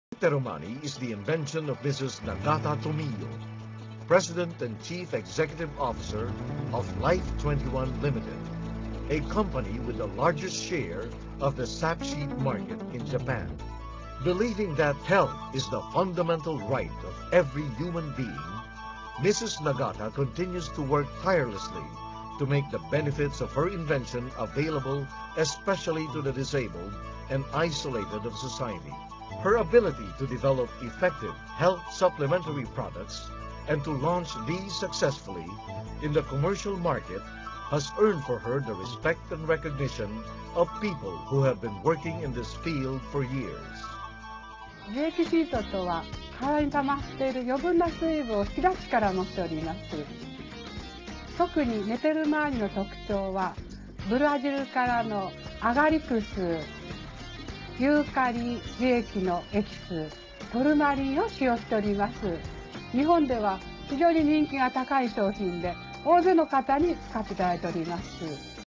Nagata Tomiyo Talks About Netterumani (Japan)
Detoxify your body comfortably and while you sleep with the Netterumani Foot Patch.